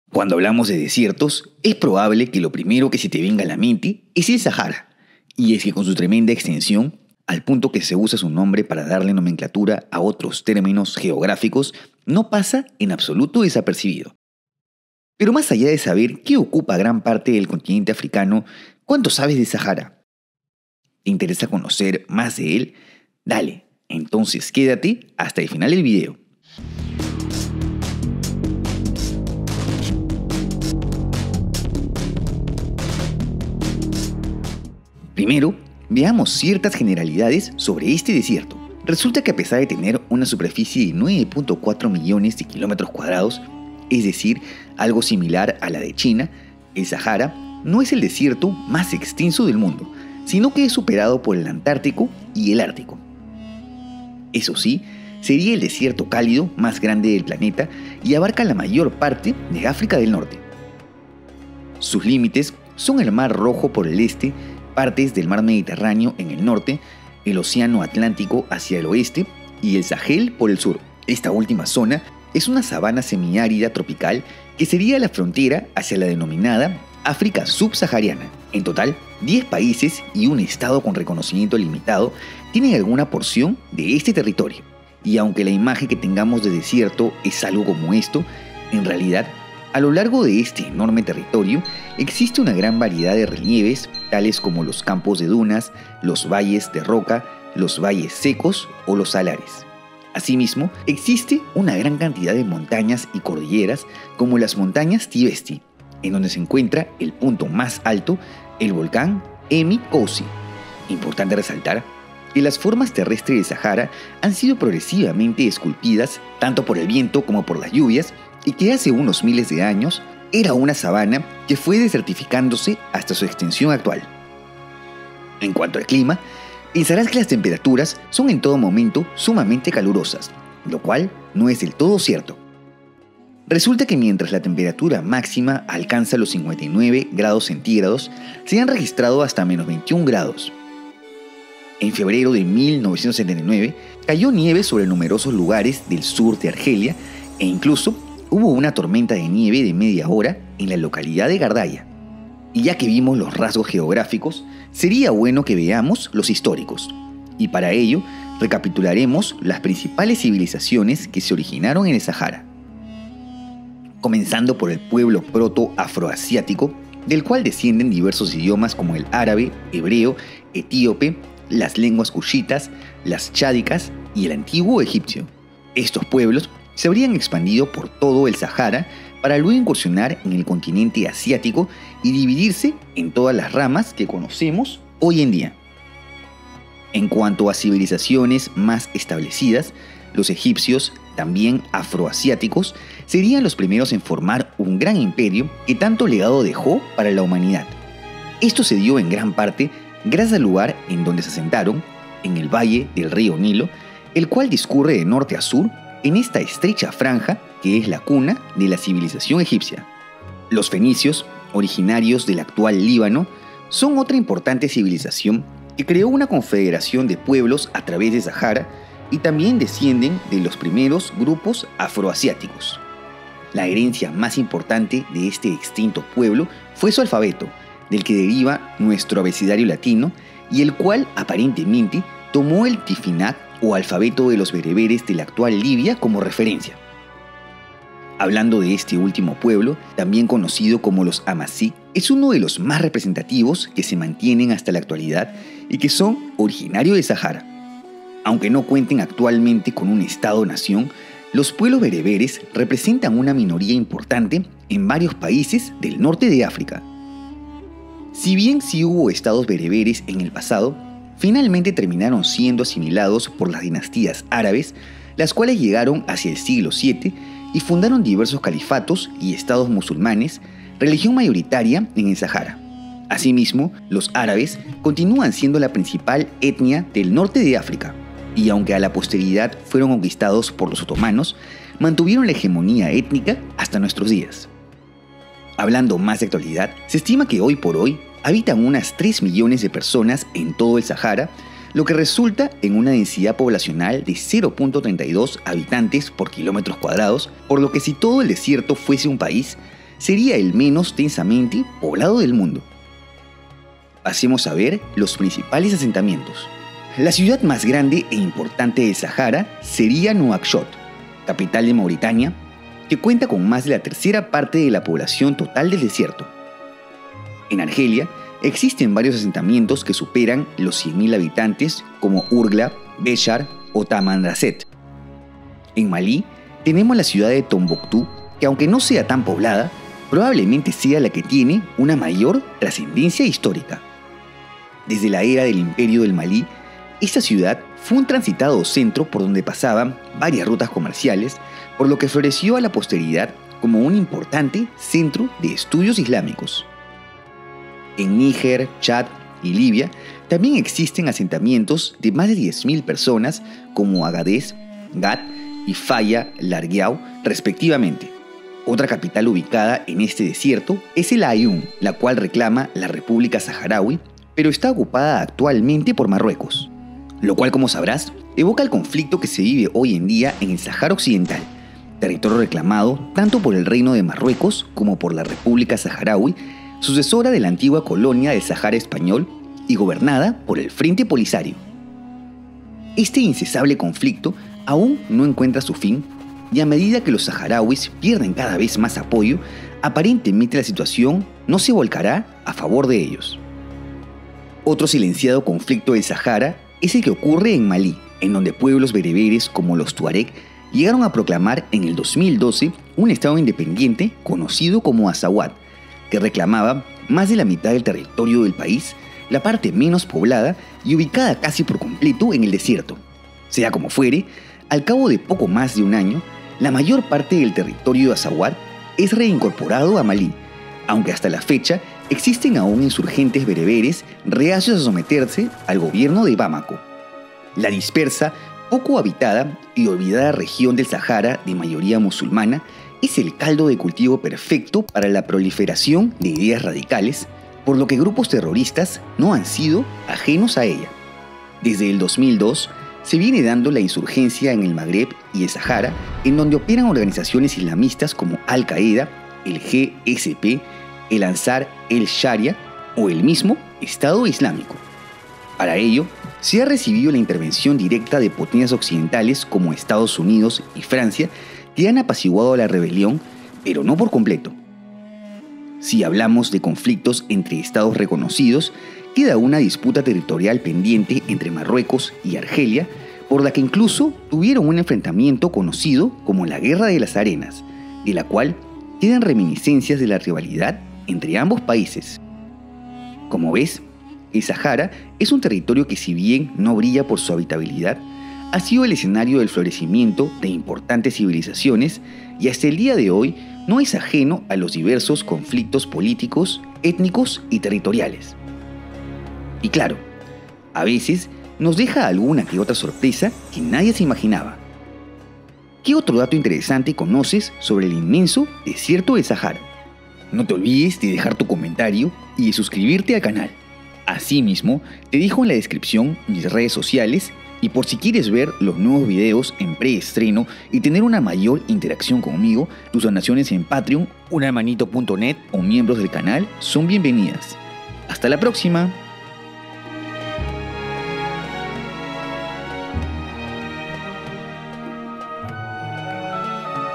¿Qué hay en el SAHARA? 🦂🐪🇩🇿🇪🇭🇲🇱 - El Mapa de Sebas
Cuando hablamos de desiertos, es probable que lo primero que se te venga a la mente es el Sahara . Y es que con su tremenda extensión, al punto que se usa su nombre para darle nomenclatura a otros términos geográficos, no pasa en absoluto desapercibido. Pero más allá de saber que ocupa gran parte del continente africano… ¿Cuánto sabes del Sahara? ¿Te interesa conocer más de él? Dale. Entonces quédate hasta el final del vídeo.

Introducción 00:00
Generalidades 00:34
Geografía y clima 01:24
Civilizaciones a través de la historia 02:32
Ciudades y población 04:45
Conflictos en el Sahara 06:07
Despedida 08:03